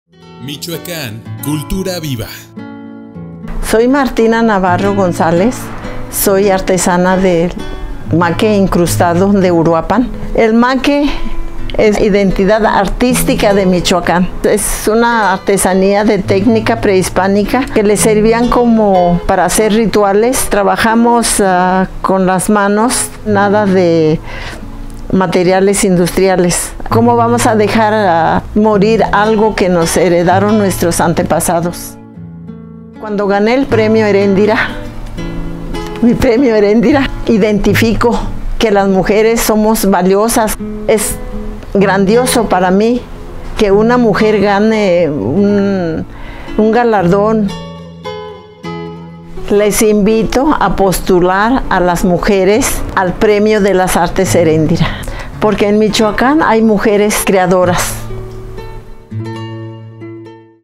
Entrevista con Martina Navarro Gonzáles galardonada al premio Estatal de las Artes Eréndira 2013
El Premio «Eréndira» impulsará de manera decidida la igualdad sustantiva, por lo que se promoverá explícitamente mayor participación de mujeres en la convocatoria, con el propósito de que más artistas michoacanas sean reconocidas con este Premio. Asimismo, el jurado será conformado con estricto apego a los principios democráticos de equidad de género.